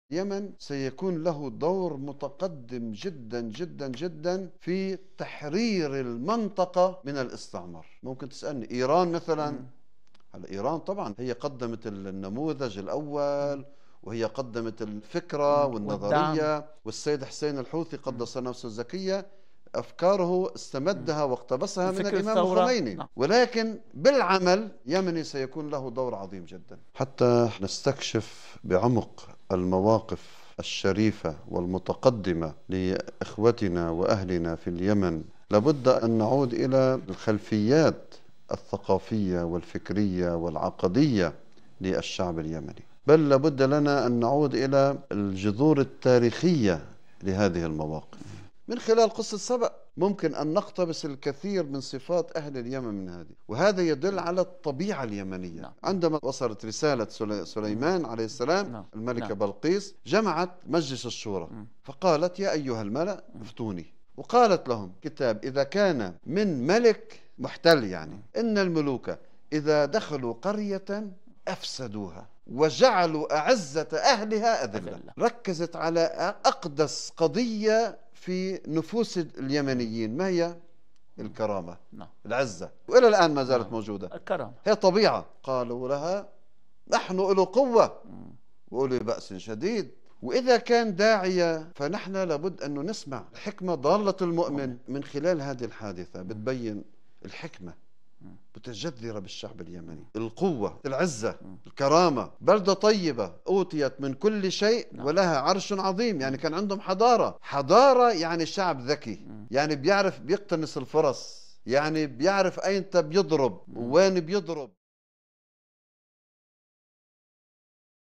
اليمن.. الرائد في تحرير المنطقة من الاحتلال | الشيخ أسد قصير ـ حفظه الله ـ
قناة أصيل الفضائية على ضوء الاسلام ومذهب أهل البيت  (عليهم السلام) وولاية الفقيه
من أهم أهدافها :

ــــــــــــــــــــــــــــــــــــــــــــــــــــــــــــــــــــــــــــــــــــــــ